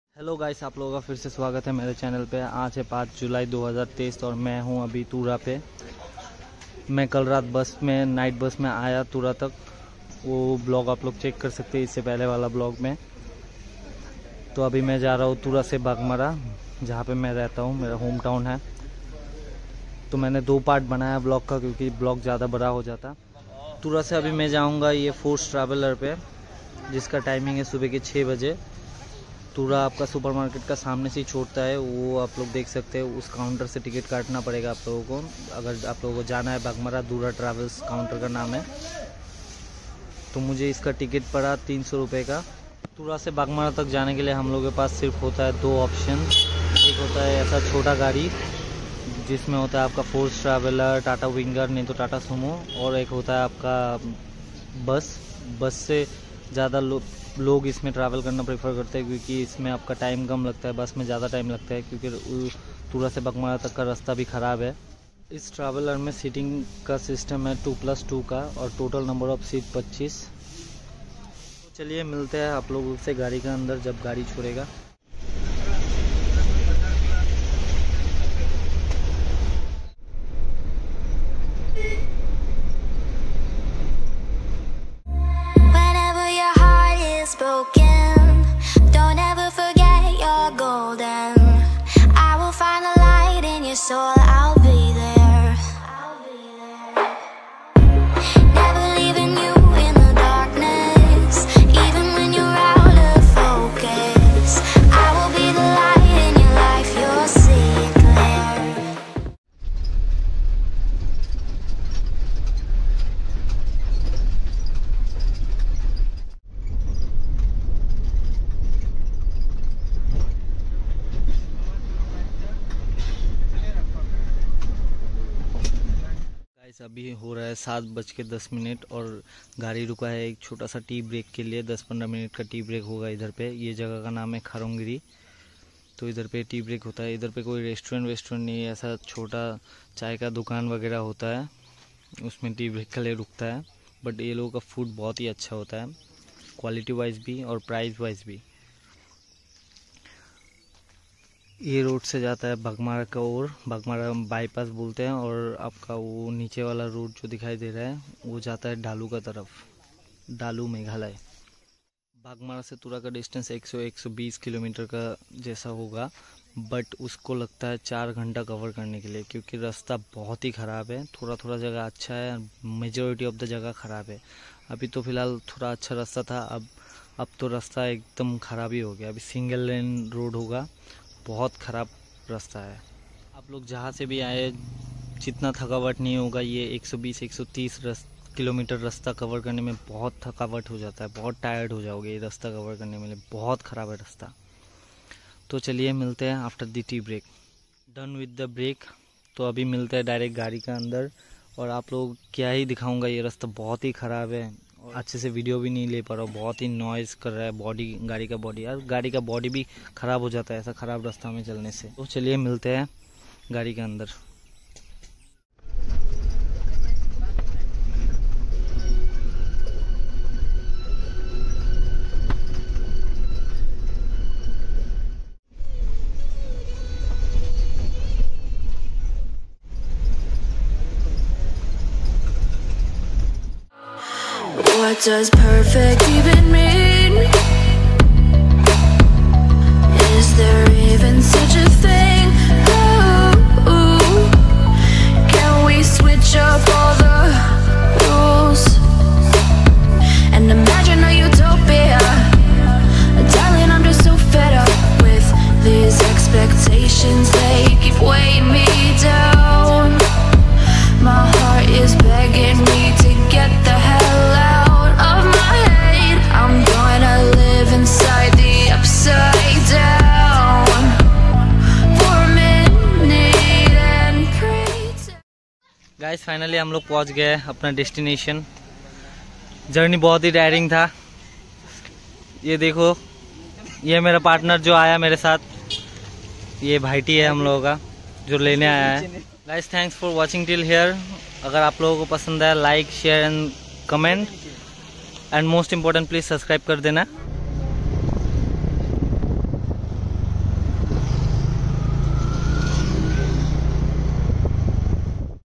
Tura to Baghmara, Meghalaya Vlog | By Force Traveller | 13th Explorer
Watch my vlog on Tura to Baghmara, South Garo Hills, Meghalaya Travelling by Salbidisha Travel's Force Traveller. This video is all about information, views and experience.

Hope you all like the vlog and if you like the vlog do:-